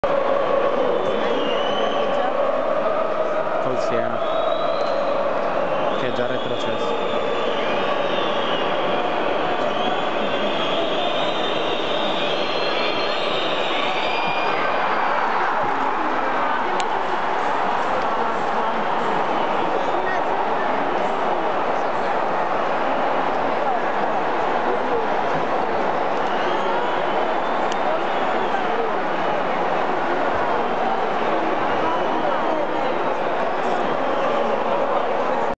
Roma Cagliari 9 maggio 2010 - il gol di Lazzari
Dalla Tevere il gol di Lazzari che ha portato in vantaggio il Cagliari